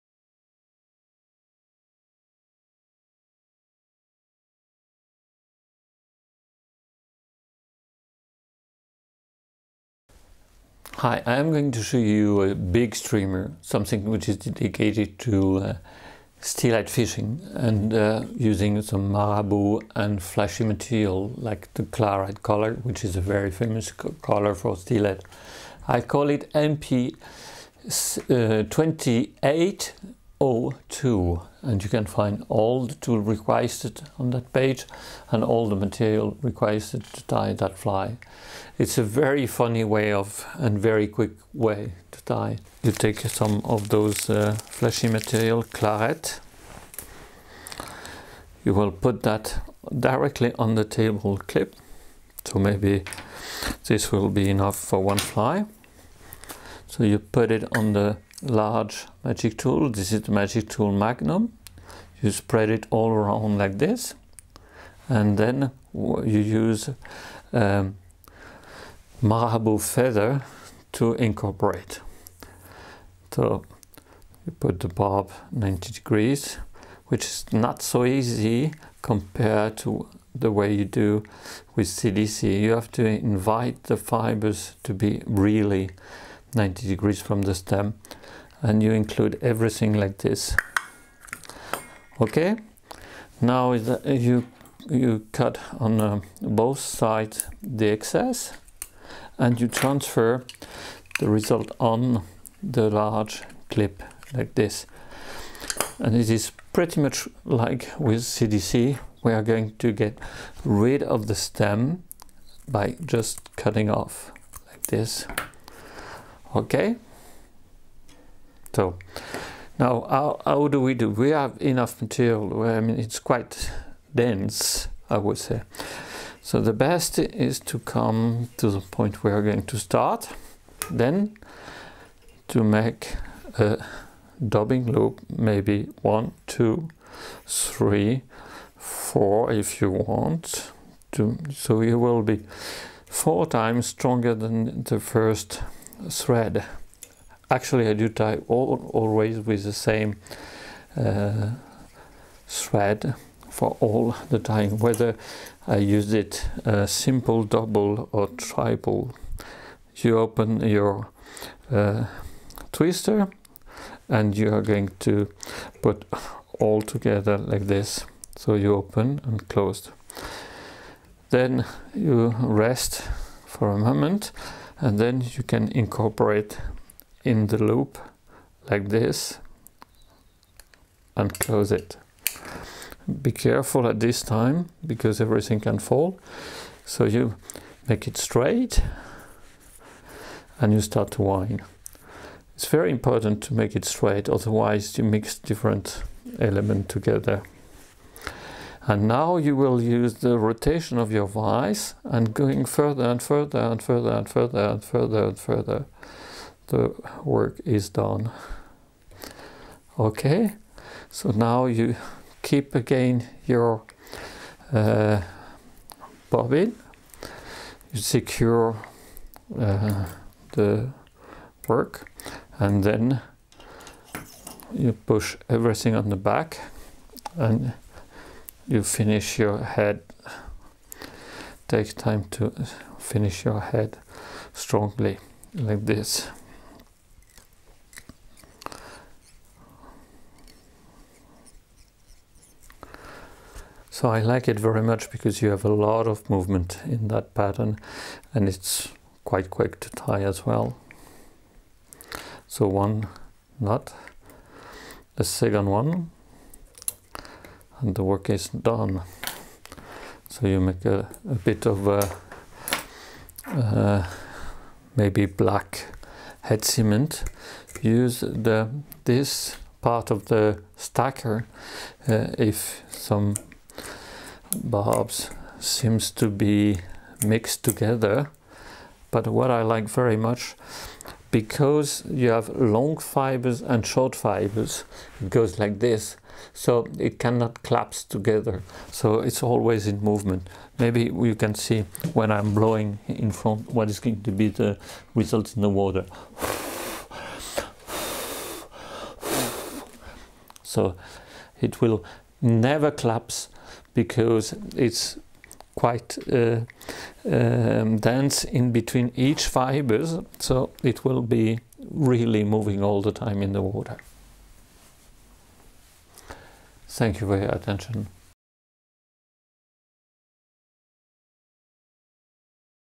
MP 2802 by Marc Petitjean
Fly tying tutorial by Marc Petitjean. Model MP 2802.
Tutoriel de montage de mouche CDC par Marc Petitjean. Modèle MP 2802.

PETITJEAN Fishing Equipment